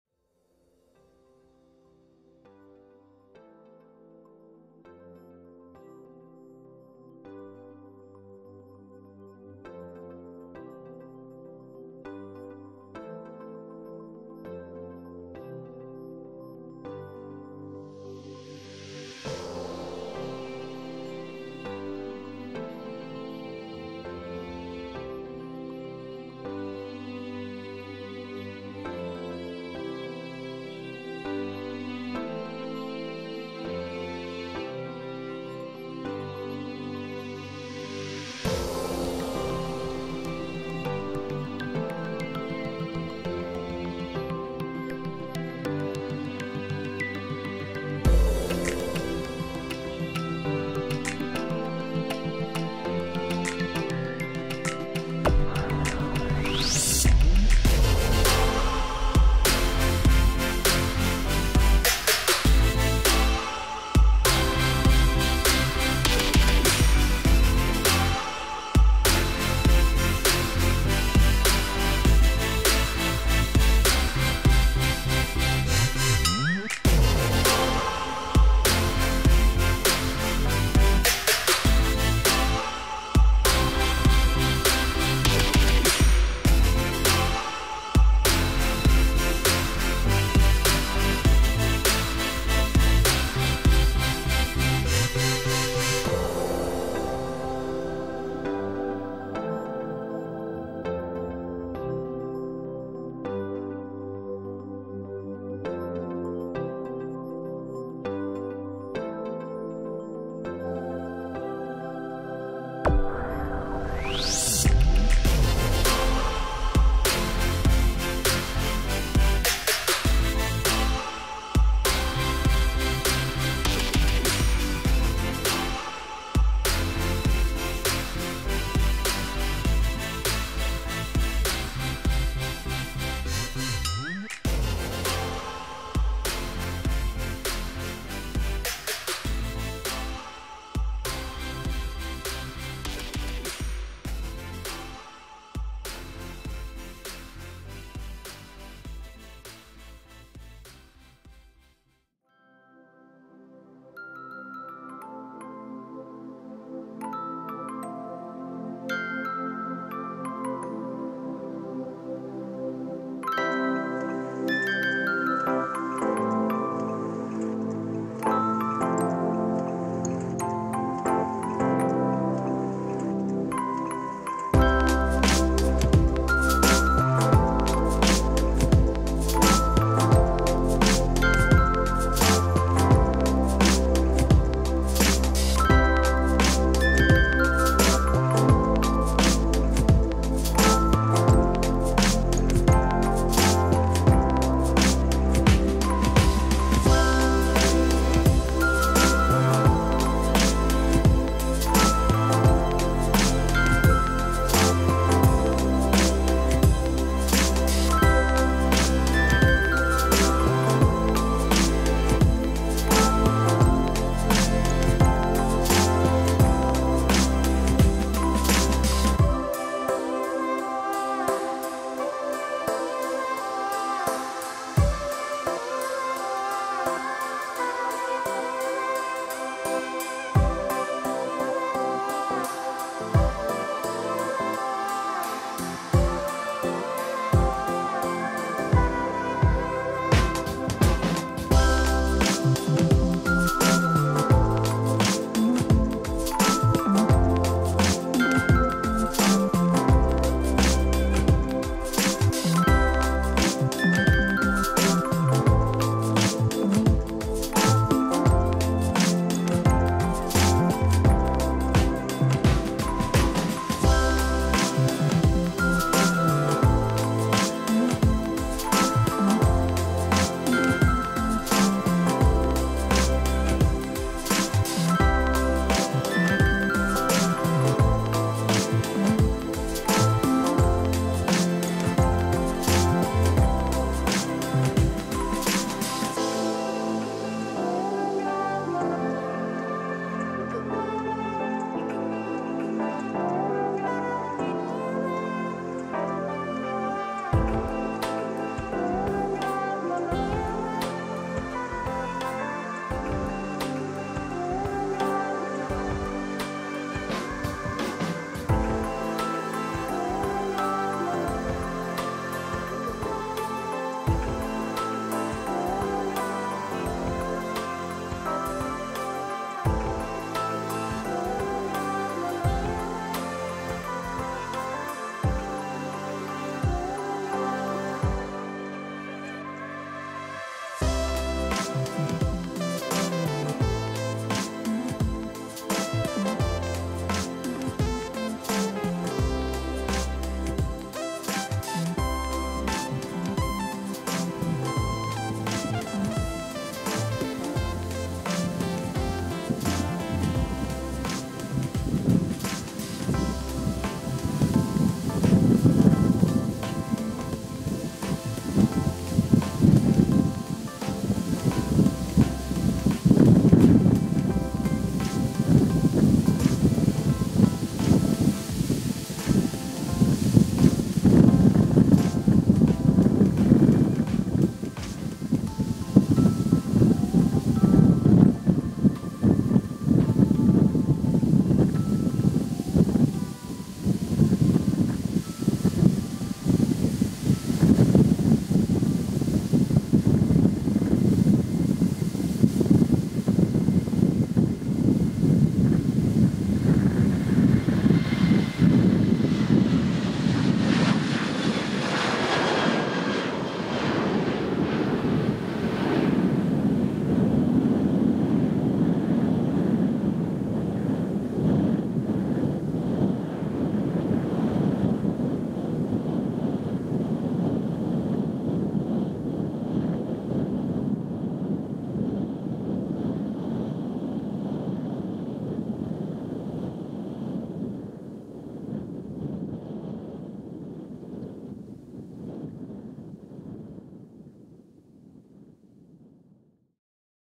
TUI Air Belgium 737
03/07/2020: Boeing 737 startup and departure at Ostend Airport.